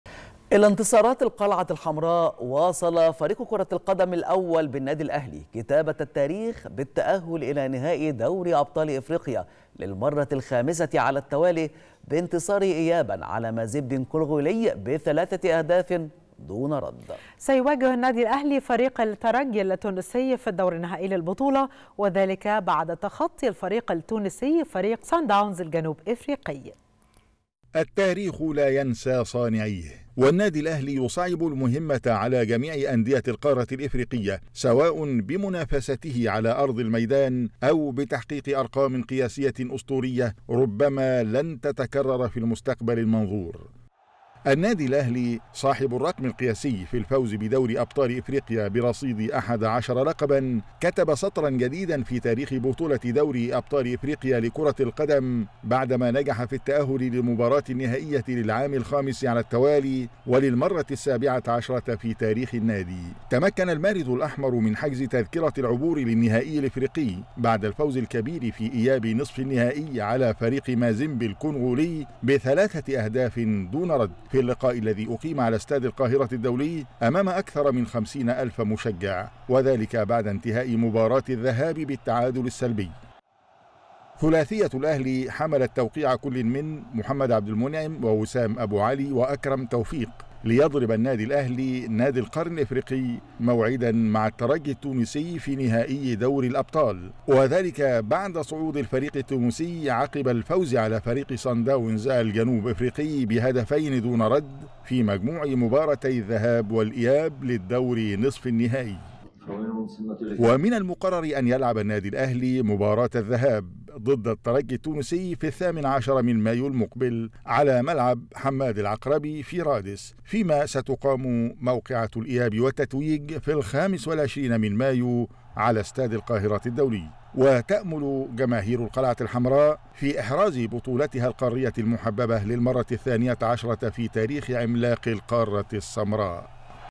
"دوري أبطال أفريقيا".. النادي الأهلي يواصل كتابة التاريخ ويتأهل للنهائي للمرة الخامسة تواليا
نشرة الثالثة | "دوري أبطال أفريقيا".. النادي الأهلي يواصل كتابة التاريخ ويتأهل للنهائي للمرة الخامسة تواليا